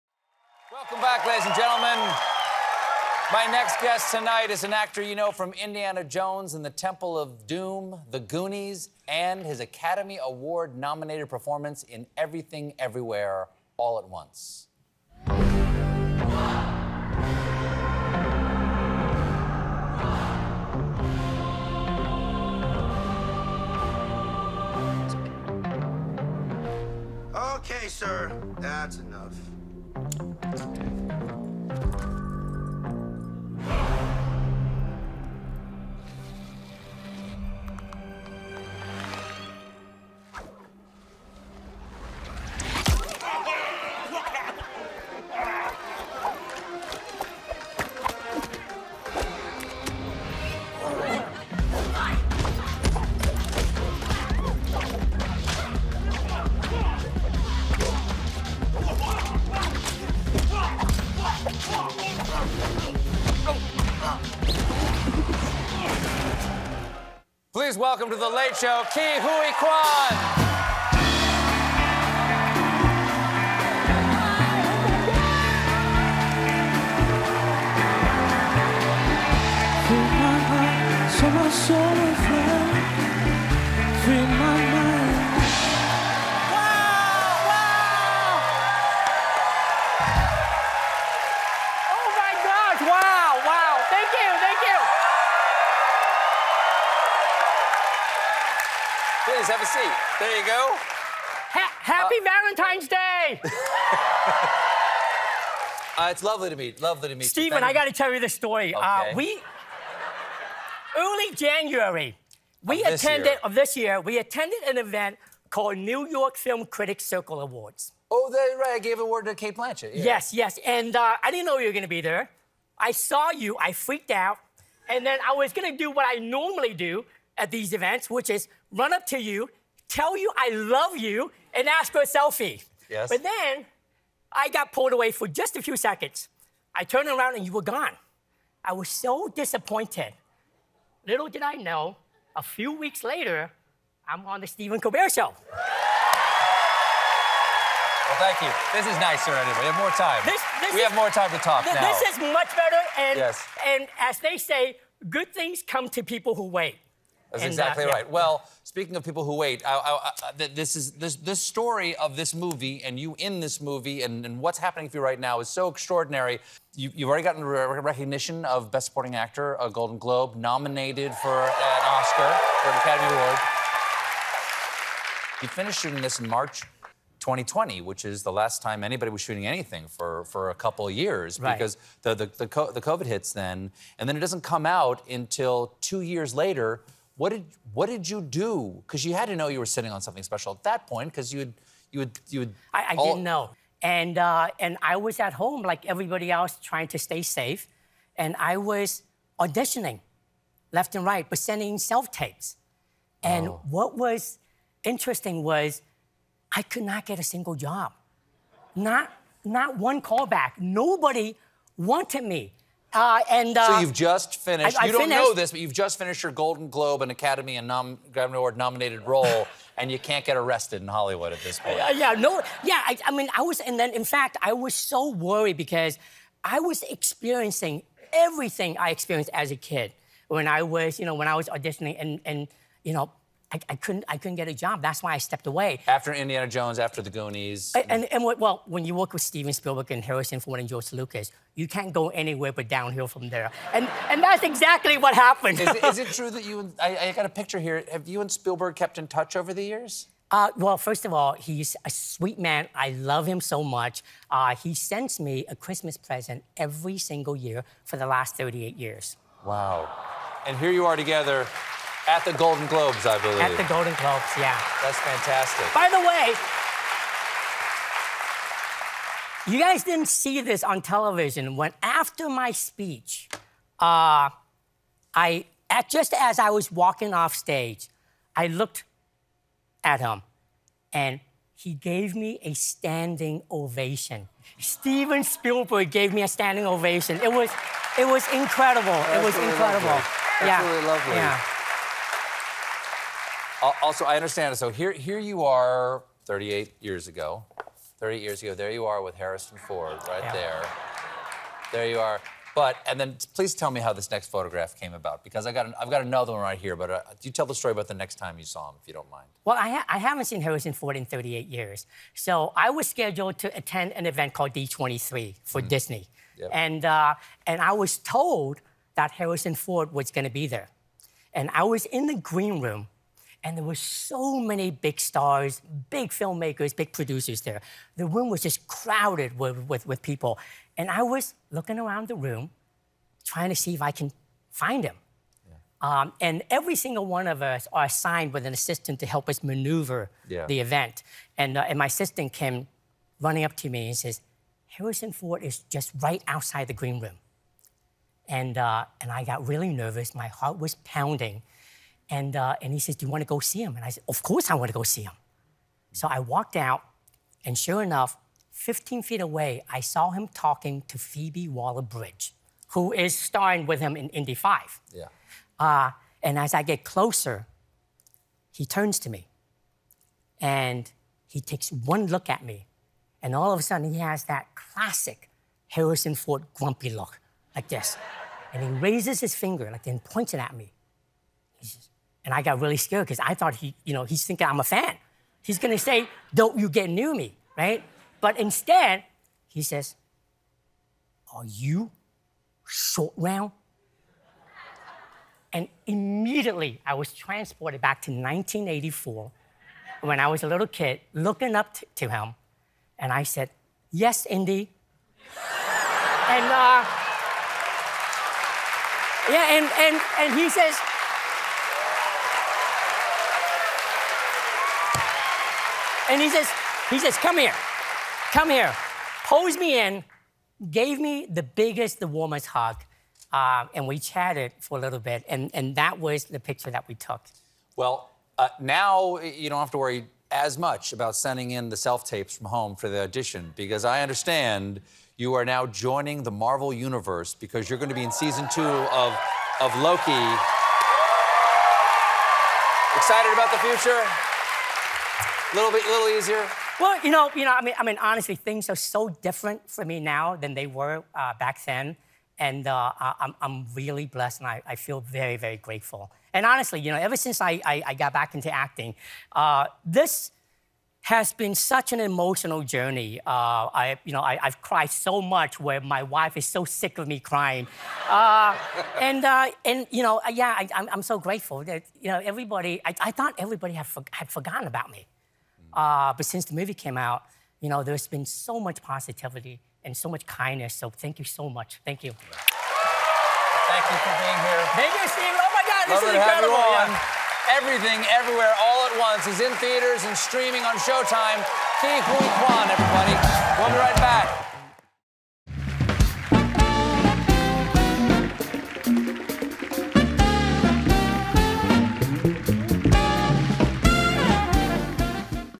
"I Thought Everybody Had Forgotten About Me" - Ke Huy Quan
Celebrated actor Ke Huy Quan tells Stephen about the emotional journey that led him to an Oscar nomination for his performance in the hit film, "Everything Everywhere All At Once," a journey which included a standing ovation from Steven Spielberg at the Golden Globes.